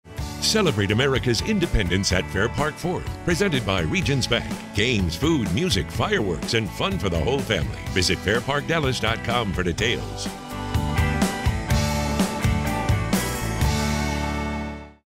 Fair Park Fourth 2023
Get ready to celebrate Independence Day in style at Fair Park Fourth, presented by Regions Bank, on Tuesday, July 4 at Fair Park!

Free and open to the public, Fair Park Fourth will feature hands-on activities for kids, local market vendors, live music, entertainment, food trucks, and other concession offerings. In addition to the fun-filled activations for all, Fair Park Fourth will light the sky with a spectacular firework show. Guests can experience the show from inside the iconic Cotton Bowl Stadium or across Fair Park.